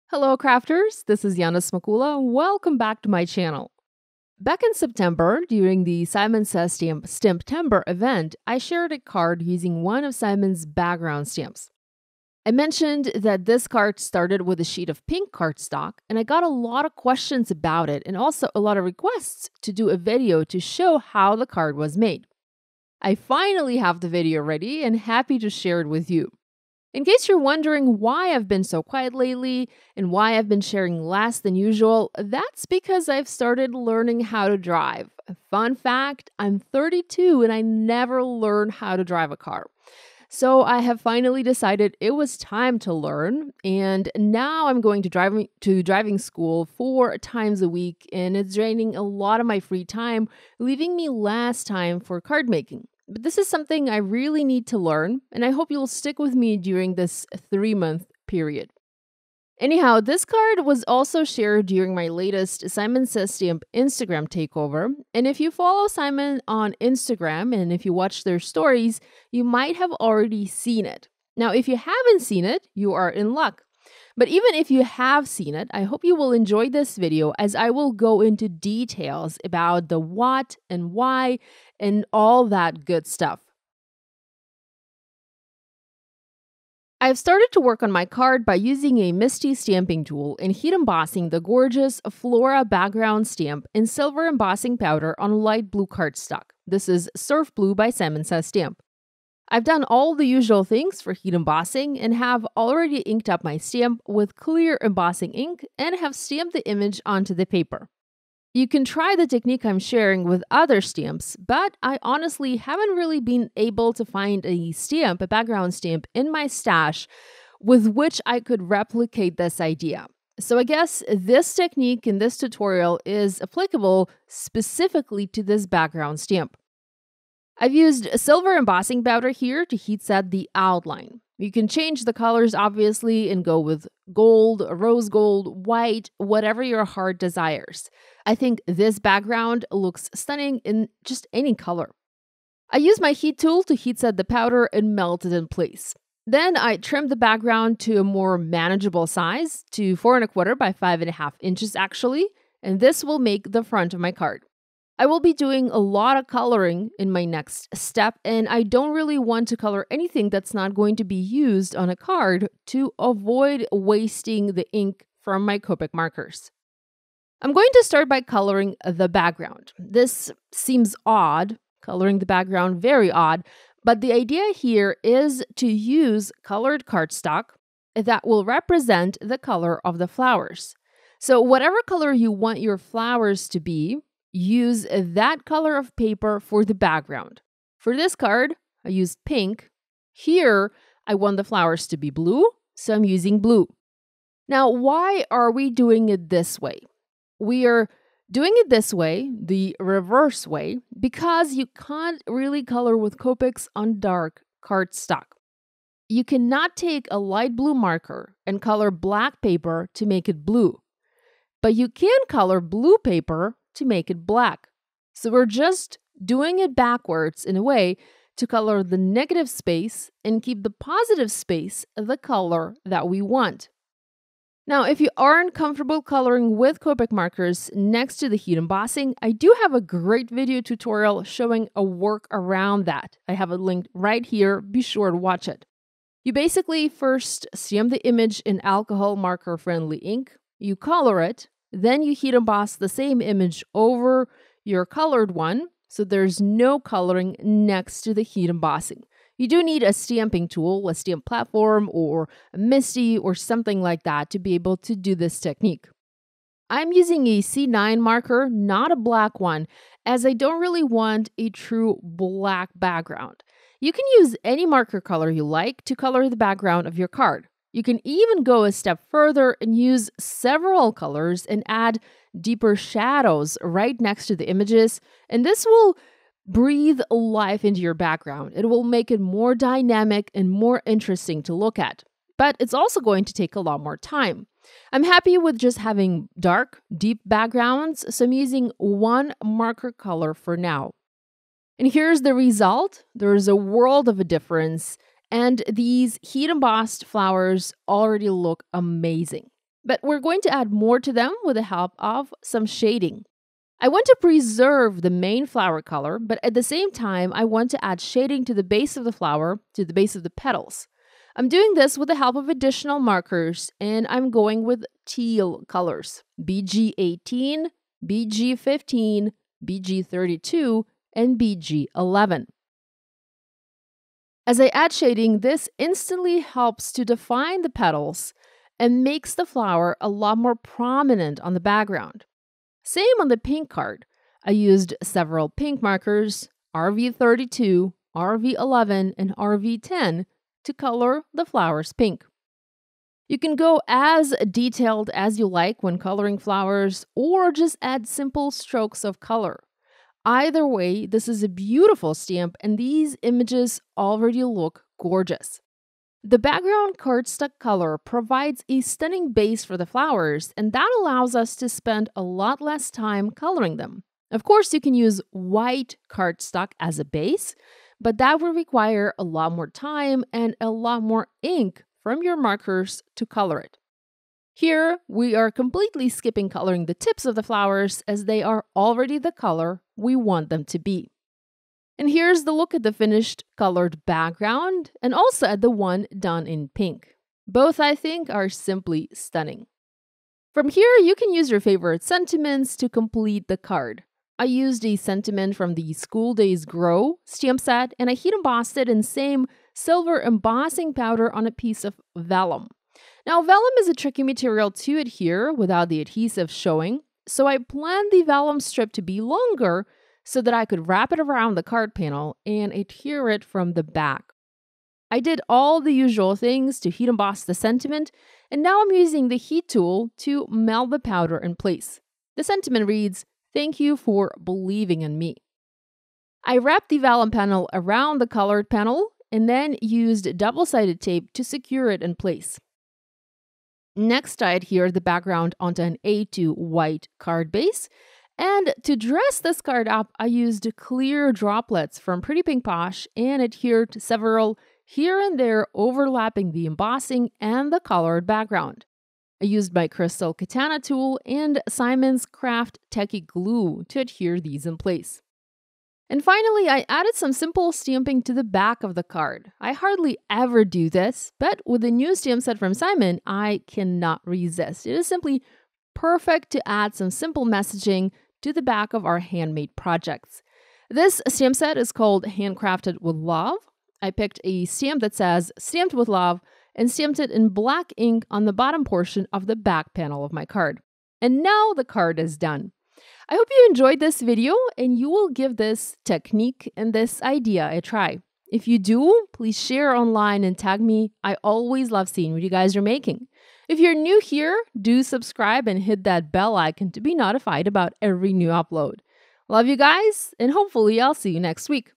Creative Coloring on Colored Cardstock
Back in September, during Simon’s STAMPtember event, I shared a card using one of Simon’s background stamps. I mentioned that the card started with a sheet of pink cardstock and I got a lot of questions about it and also a lot of requests to do a video to show how the card was made.

I finally have the video ready and happy to share it with you! This card was also shared during my latest Simon’s Instagram Takeover, and if you follow Simon on Instagram and watch their stories you might have already seen it. If you have haven’t – you are in luck! But even if you have – I hope you’ll enjoy this video as I go into details about that why and what’s and all that good stuff.